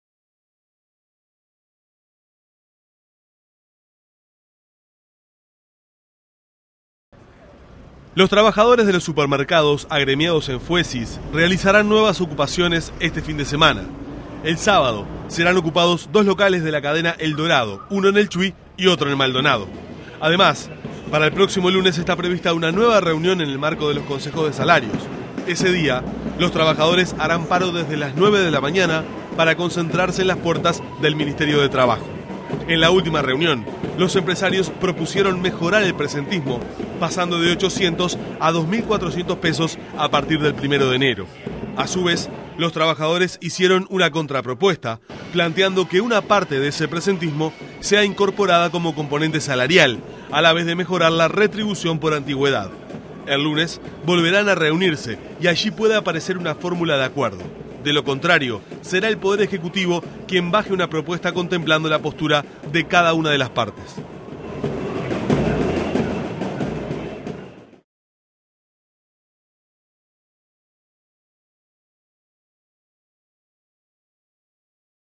Fuecys vuelve a ocupar supermercados por mejores salarios
Los trabajadores de los supermercados volverán a ocupar locales este fin de semana en Rocha y Maldonado. Además, el lunes harán paro desde las 9 de la mañana para acompañar las negociaciones en los consejos de salario.

Los trabajadores de los supermercados agremiados en Fuecys realizarán nuevas ocupaciones este fin de semana. El sábado será el turno de dos locales de la cadena El Dorado, uno en Chuy y otro en Maldonado.

Para el próximo lunes está prevista una nueva reunión en el marco de los Consejos de Salarios. Ese día, los trabajadores harán un paro desde las 9 de la mañana para concentrarse en las puertas del Ministerio de Trabajo.

En la última reunión, los empresarios propusieron mejorar el presentismo, pasando de 800 a 2.400 pesos a partir del 1º de enero. Los trabajadores hicieron una contrapropuesta, planteando que una parte de ese beneficio sea incorporada como componente salarial, a la vez de mejorar la retribución por antigüedad.

El lunes volverán a reunirse y allí puede aparecer una fórmula de acuerdo. De lo contrario, será el Poder Ejecutivo quien baje una propuesta contemplando la postura de cada una de las partes.